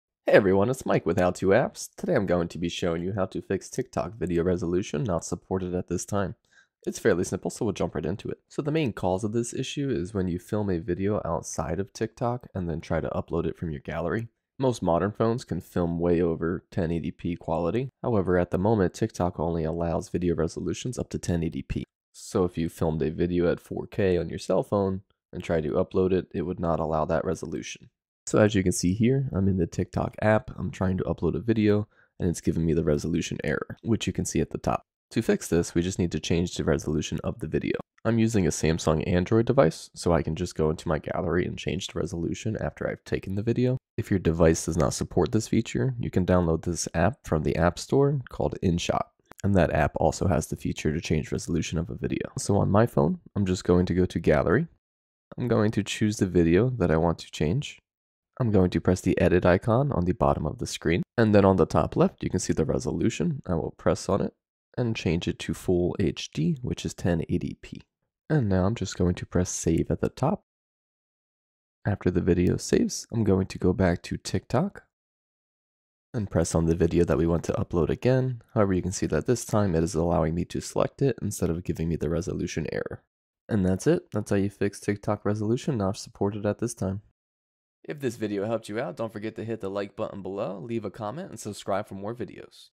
How To Fix TikTok Video Resolution Not Supported At This Time
Let's show you how to fix the message "TikTok video resolution not supported at this time".

In this video, I walk you through the steps to fix the error message: "TikTok video resolution is not supported at this time. The main cause of this issue is when you film a video outside of TikTok and then try to upload it to TikTok. Most cameras can film over the resolution that TikTok allows. To fix this, change the resolution of the video. If your Android or Apple device cannot change the video resolution then I recommend downloading InShot from the Store and using that app. On my Samsung, I go to Gallary, select the video, click on Edit and then click on the resolution at the top left and select FHD and then Save. Now I can upload it to TikTok.